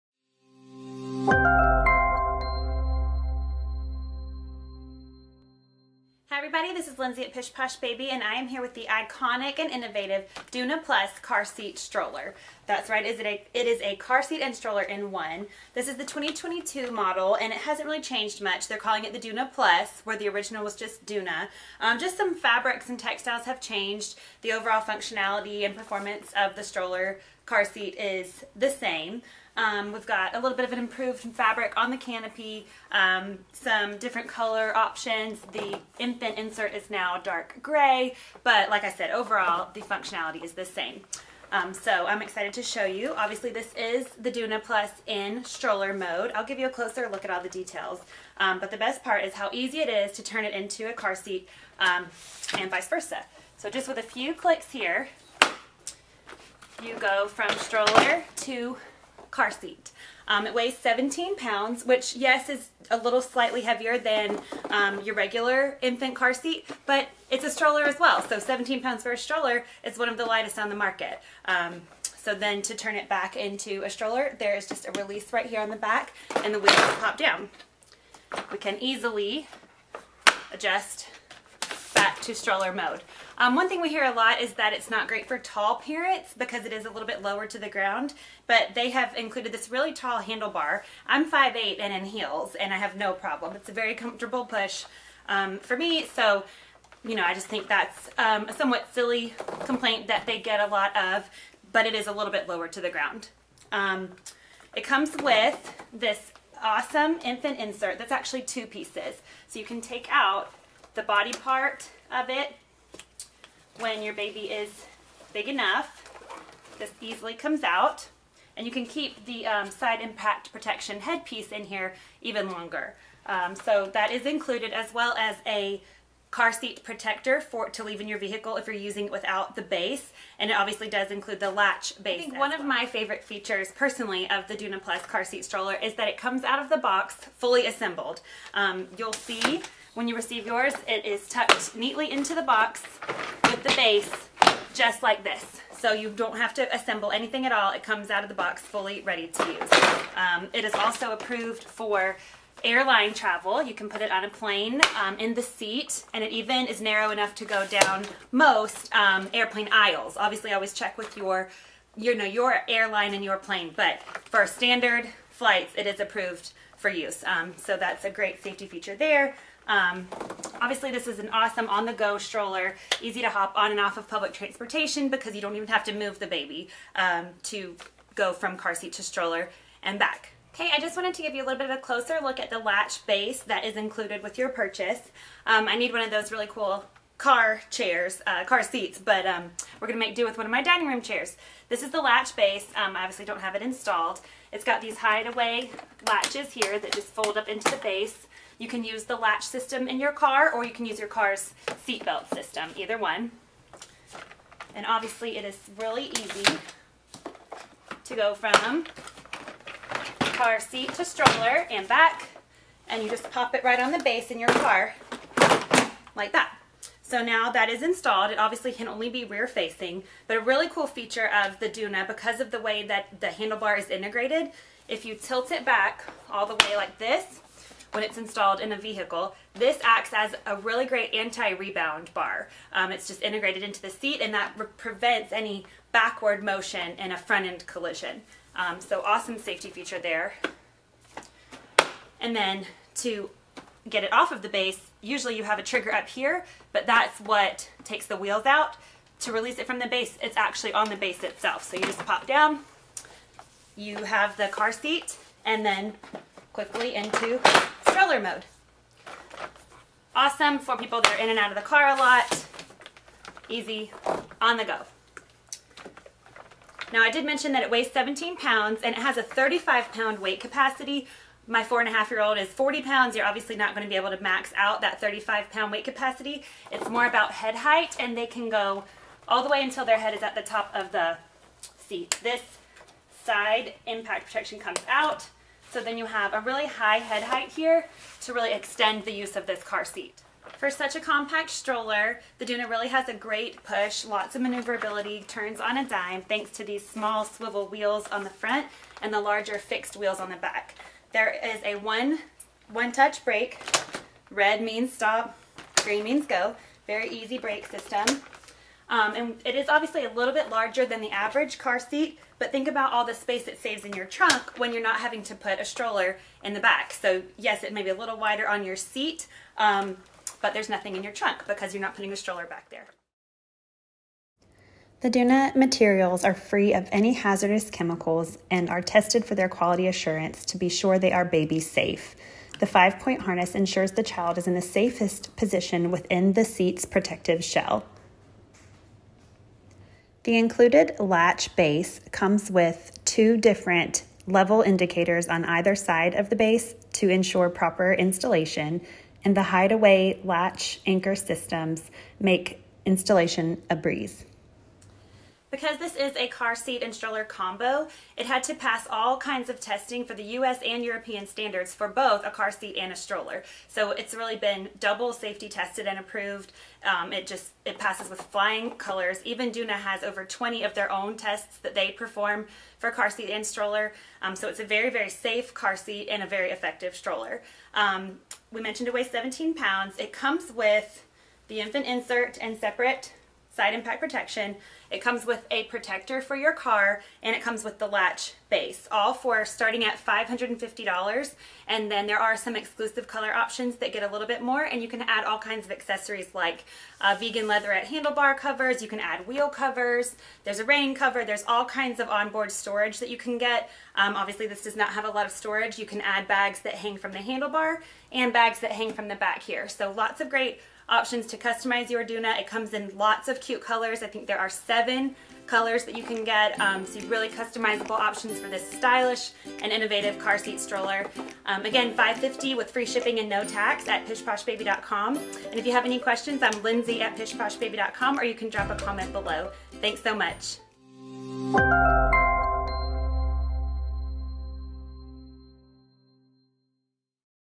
Full Review of Doona Infant Car Seat Stroller
Lindsey gives us an in depth tour of the innovative Doona+ Car Seat Stroller. It goes from car seat to stroller and back in seconds! Take a look!